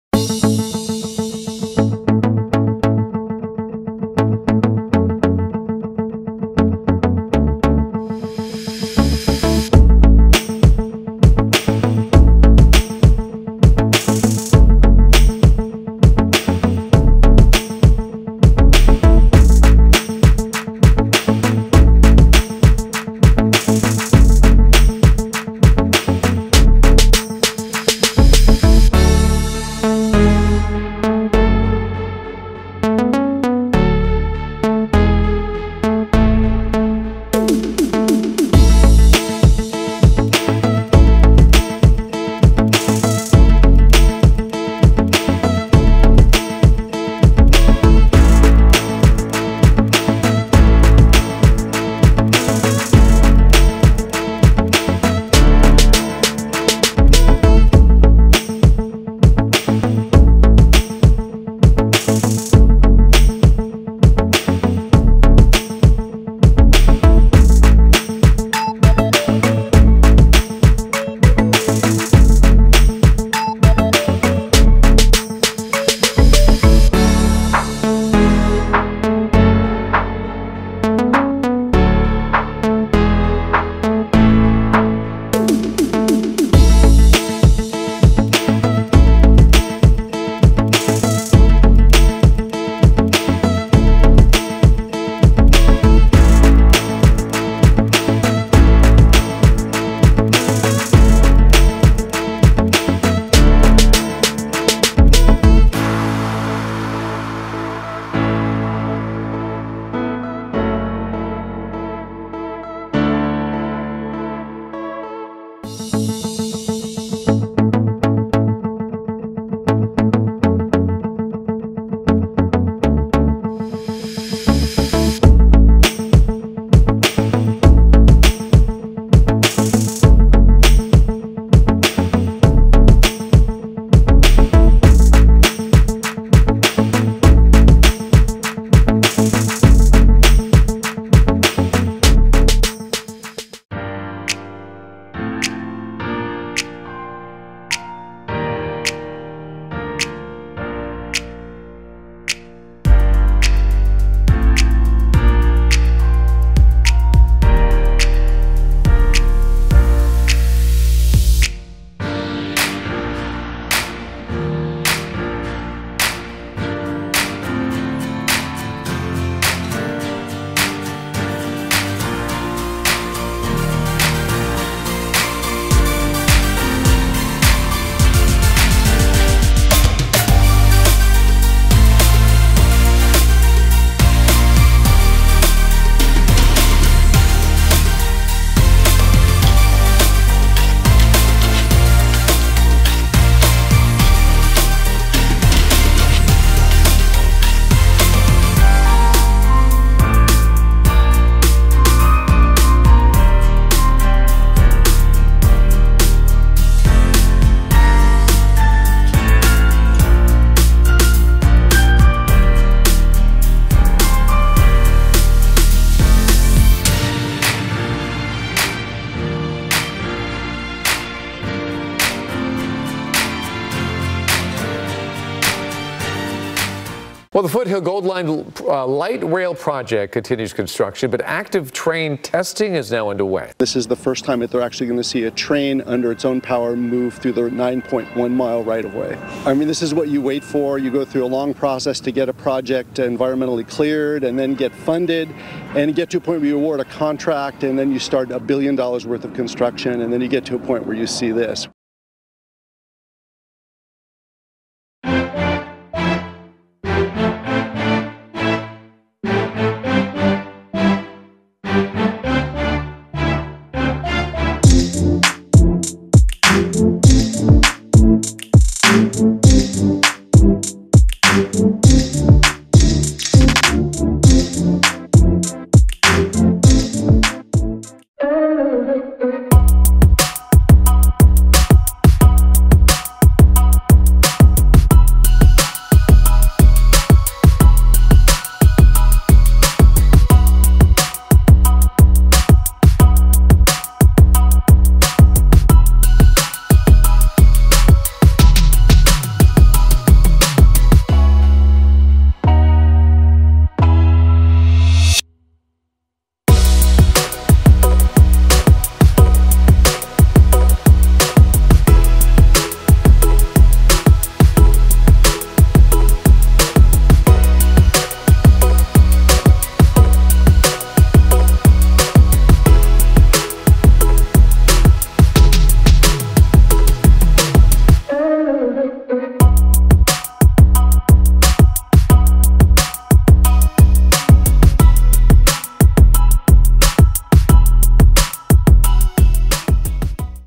Foothill Gold Line Project Update Highlights - July 2024
Chapters:
00:04 Stations
02:24 Train Testing
04:16 Pomona to Montclair
04:38 In the Community